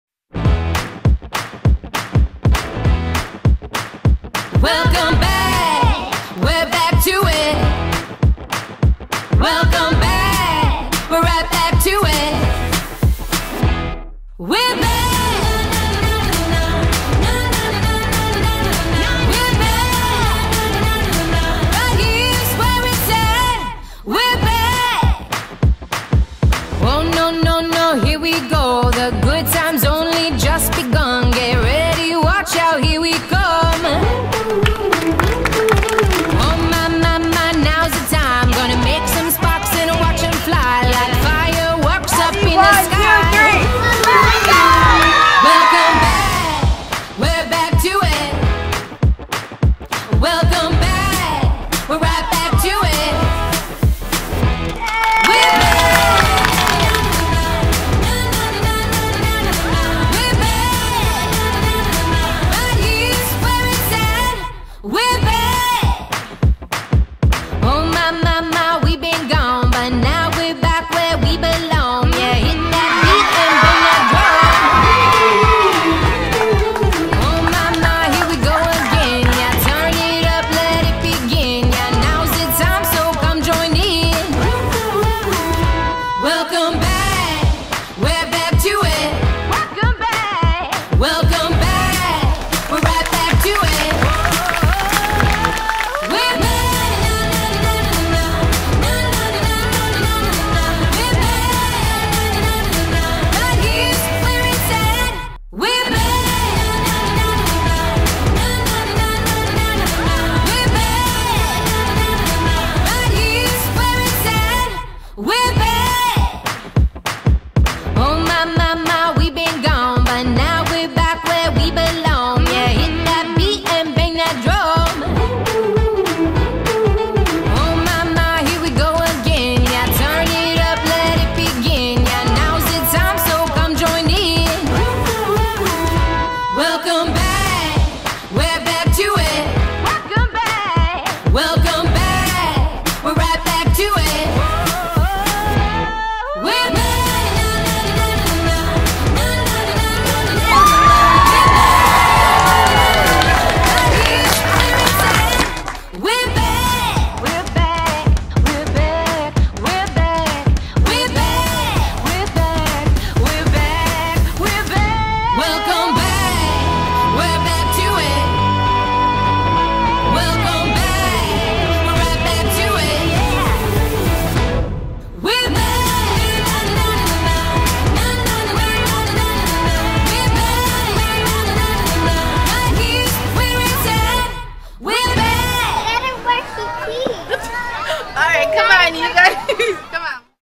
2021 Library Summer Programs--Blades is back!
As part of the partnership between the Boston Bruins and the Massachusetts Board of Library Commissioners, Bruins mascot Blades makes special library visits to promote reading over the summer